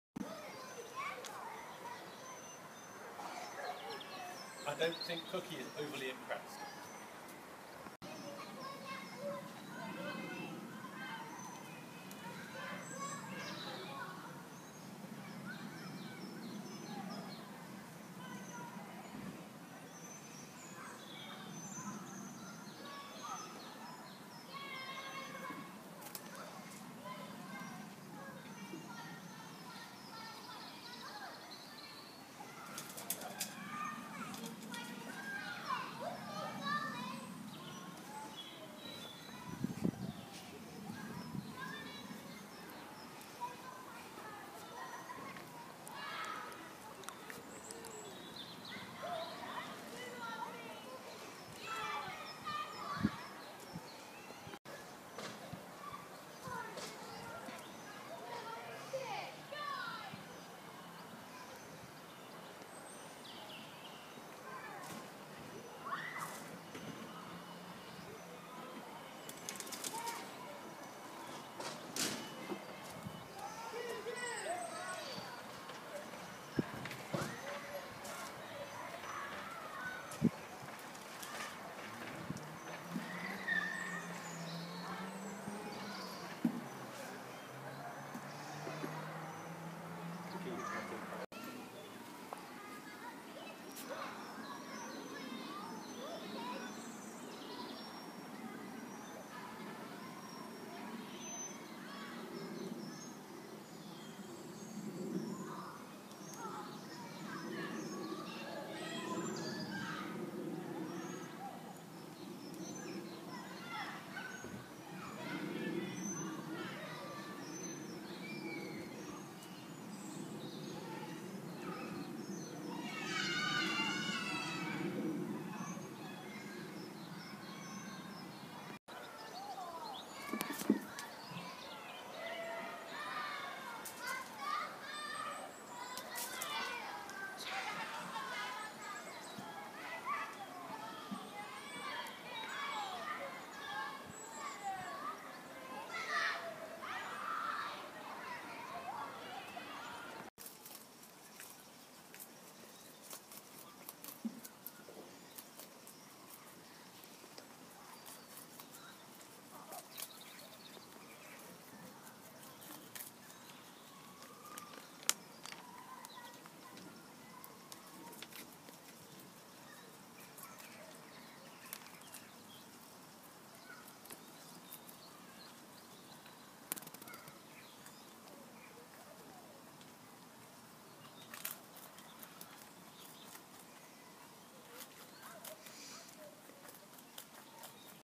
Rabbits Noel and Alfie speed around the garden - amazing!
Annabelle filmed this pair of brothers running like mad things in her garden as she is currently fostering them.  I suspect they will not want to come back ...